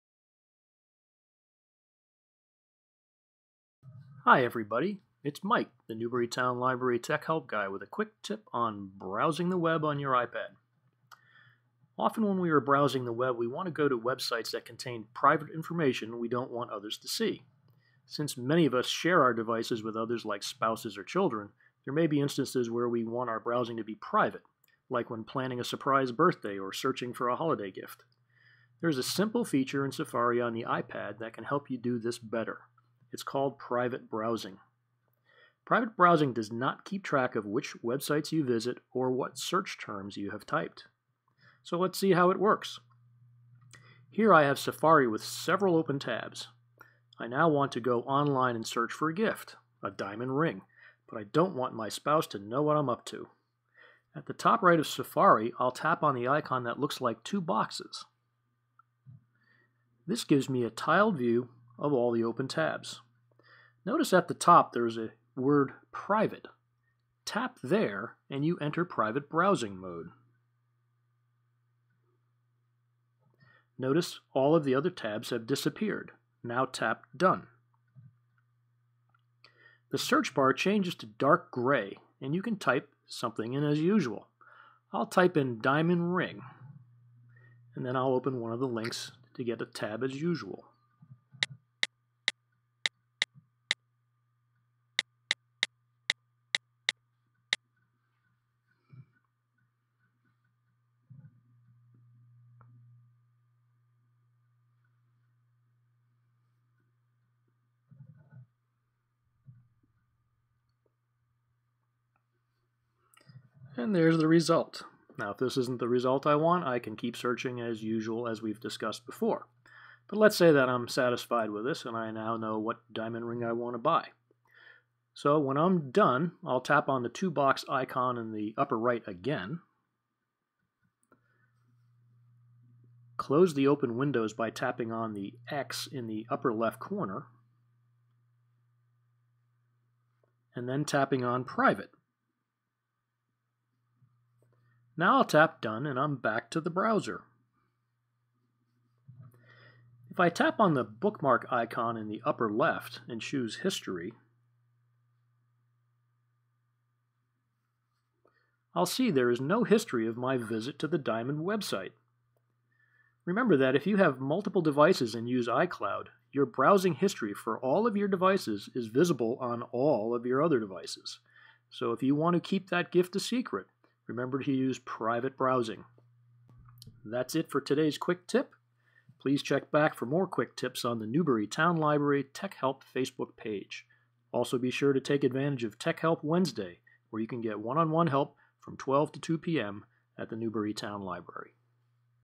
Private Browsing in Safari on iPad
This quick tip shows you how to open Private Tabs in Safari on the iPad